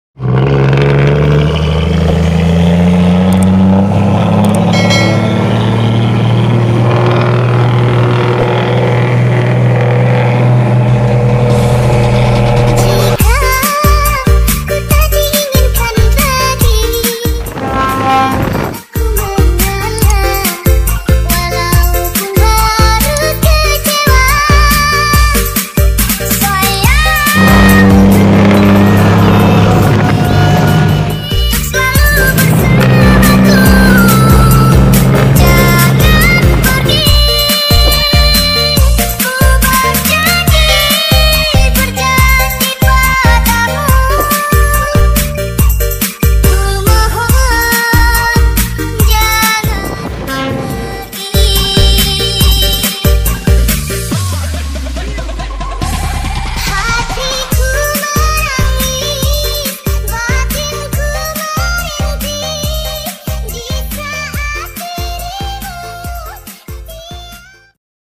SELEBRITIS LINTASAN KALSEL-KALTIM TAMPIL DENGAN PESONA SUARA KNALPOT BUS PULAU INDAH SAMARINDA#TRUK
kembali lagi kami tayangkan pemain fuso kalsel yang berada di lintasan kaltim dengan penampilan yang begitu sederhana ,tetapi tidak kalah suara knalpot dengan bus pulau indah samarinda , sangat persis dengan bus nya pulau indah . jadi banyak cctv jalan raya di manapun bertemu dengan nya banyak yang mengabadikan momen suara knalpotnya . di situlah orang banyak kenal dengan nya

jadi itulah penjelasan sedikit tentang  fuso lintasan kalsel-kaltim

Salam.  TRUK KALSEL CHANNEL👍👍👍